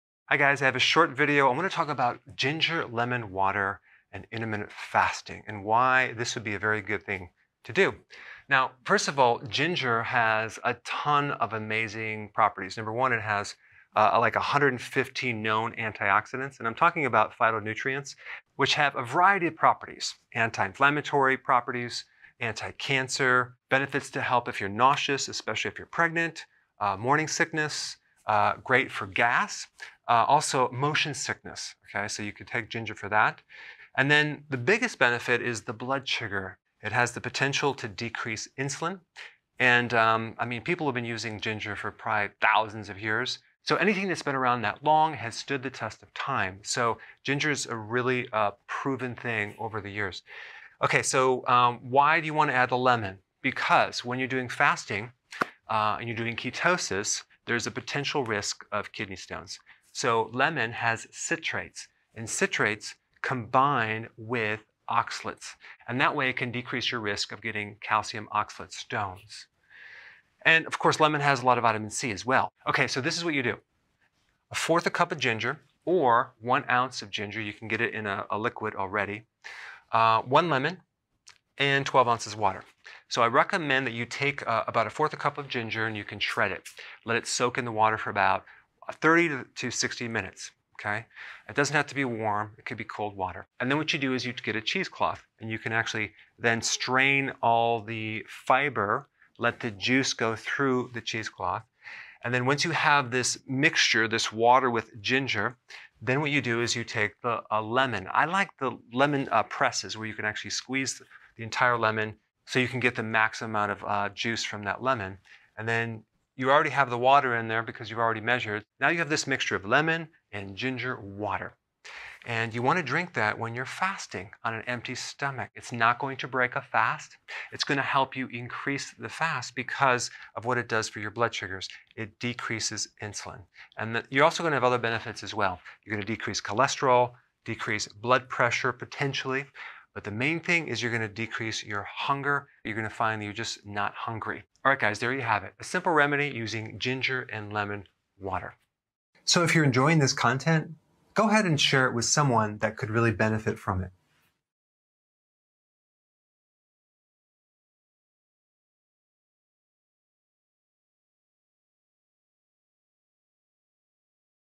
ИНТЕРВАЛЬНОЕ ГОЛОДАНИЕ: как помогает имбирно-лимонный напиток?
Имбирь имеет уникальные свойства. Он помогает на интервальном голодании. Рецепт приготовления имбирно-лимонного напитка. Что в нем полезного и как он влияет на здоровье и организм. Почему важно пить его регулярно.